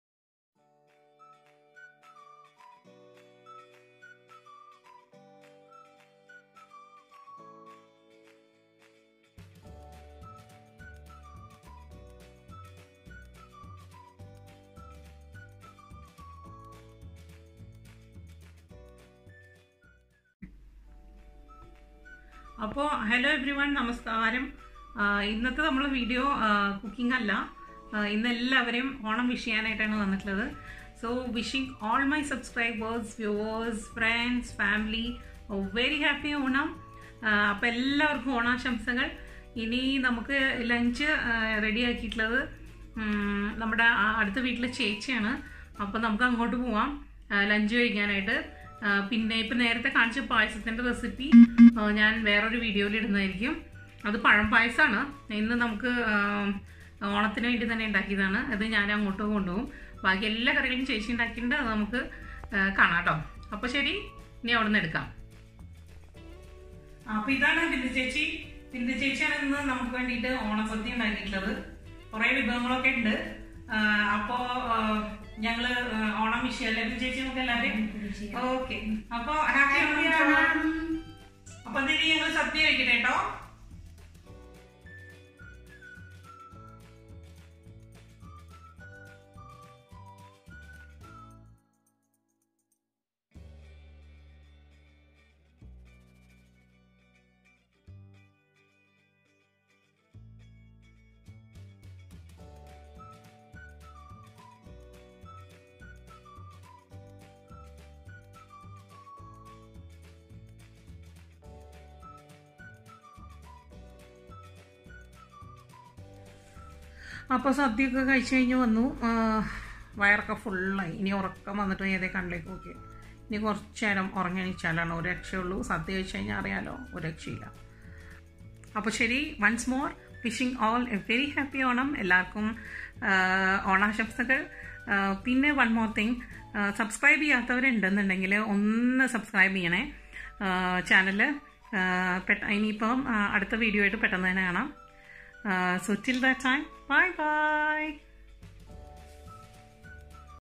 Onam with Bindu Chechi | ബിന്ദു ചേച്ചിയുമായി ഒരു അടിപൊളി ഓണം | Onasadya | Pookalam
Even though we NRI's miss the fun that we have at Kerala for Onam,  we try to be happy with what we are blessed with.  Me and Neha having special Onam Lunch (ഓണസദ്യ ) with our neighbour in Dubai.

The saying very apt  - be happy with what you have (ഉള്ളതുകൊണ്ട് ഓണം പോലെ)

We had a fantastic Onam today. Thanks to Bindu   Chechi for all the efforts.

അങ്ങനെ ഒരു സന്തോഷപ്രദമായ ഓണവും കഴിഞ്ഞു. അല്ഹമ്ദുലില്ല...

നിങ്ങളുടെ ഓണാഘോഷം എങ്ങിനെ ആയിരുന്നു? How was your Onam celebrations?

Please share with us in comments.  കമെന്റ് ആയി ഷെയർ ചെയ്യണേ...